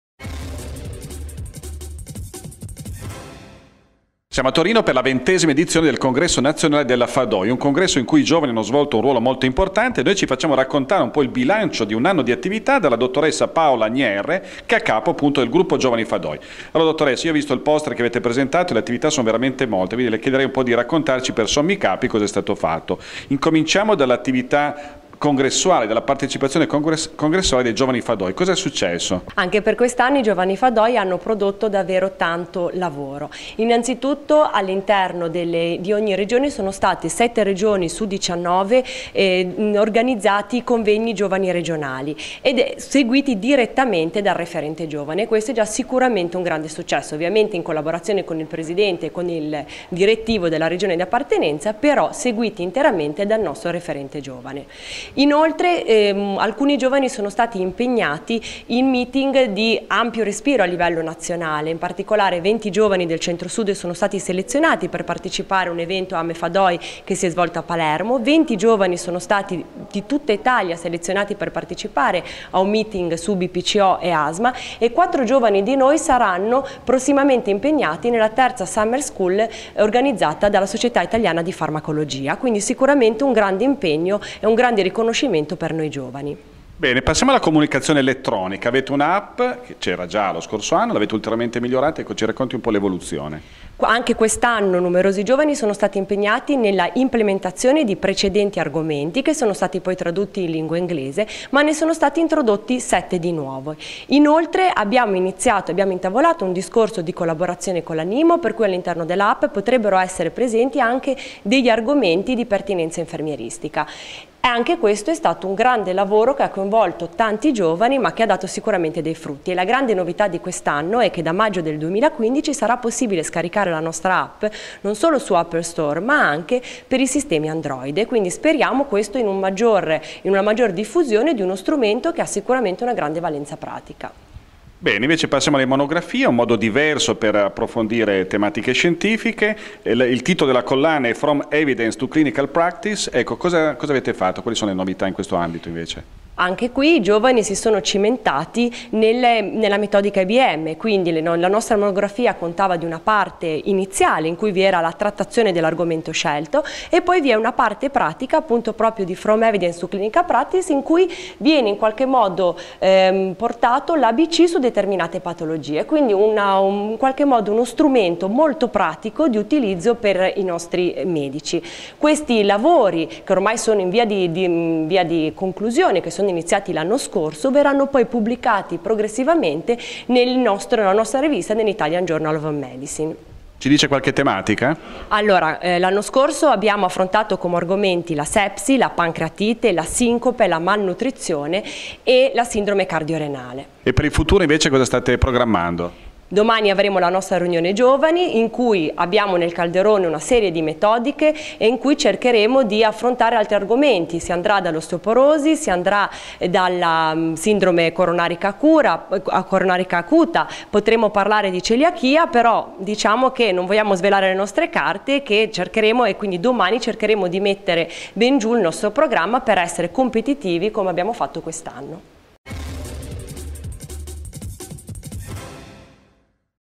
Giovani internisti FADOI: bilancio di un anno di attività
Intervista alla Dr.ssa Paola Gnerre, Coordinatore Nazionale Commissione Giovani FADOI